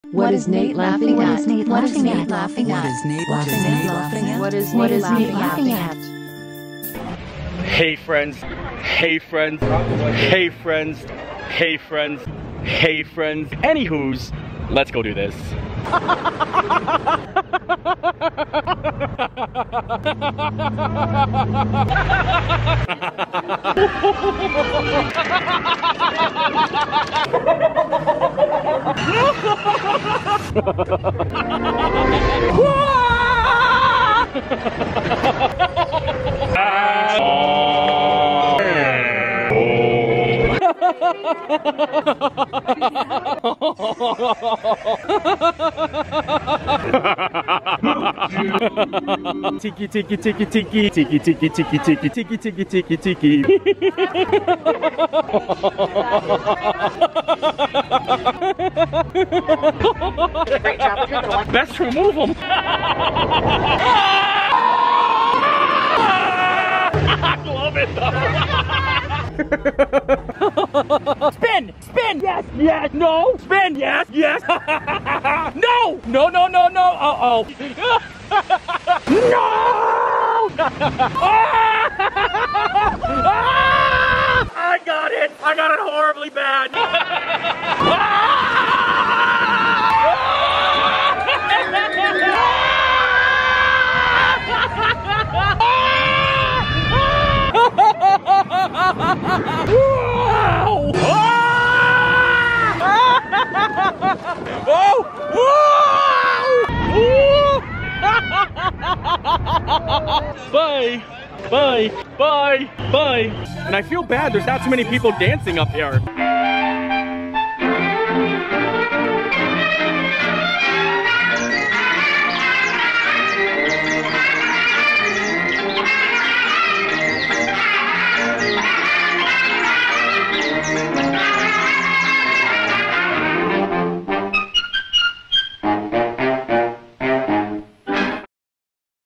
What is Paging Mr. Morrow laughing at?
Paging Mr. Morrow sure seems like a jolly fellow. Let's take a break from the heaviness of the Kay Bros. and laugh along with Nate!